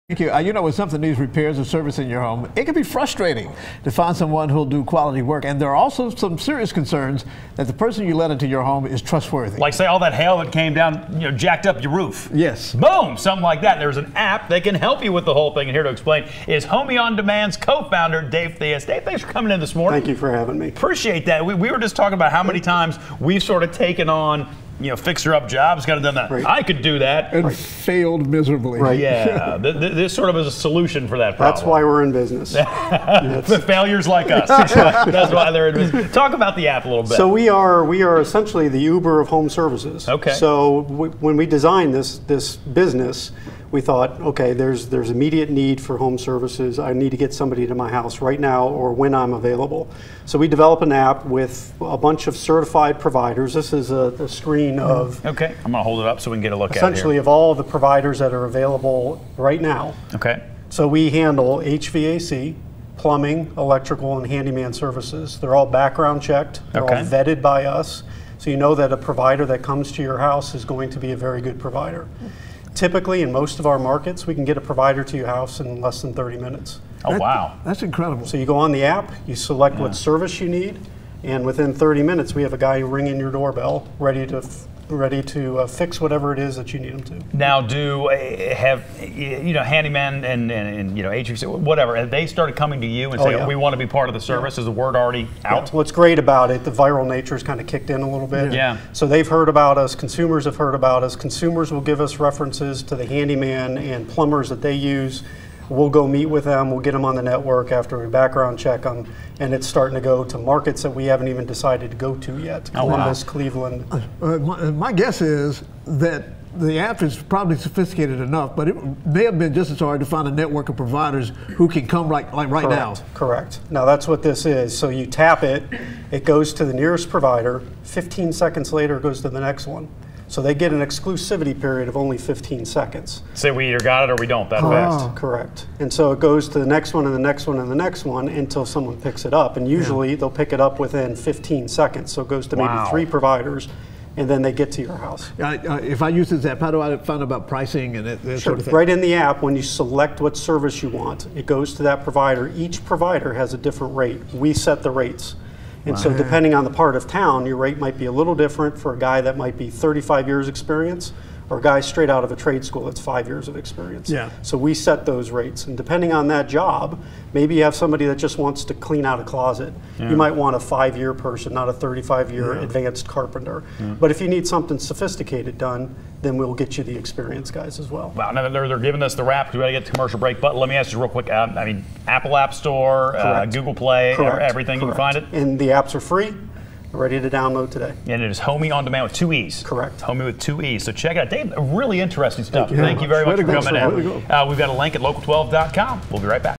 New app can make home repairs easier
CINCINNATI (WKRC) - When something needs repairs or service in your home, it can be frustrating to find someone who will do quality work. There are also concerns that the person you're letting in your home is trustworthy.

But now, there's an app that can help with that.

CTO and co-founder of Homee On Demand Dave Theus joined us in the studio Monday to tell us more about the app.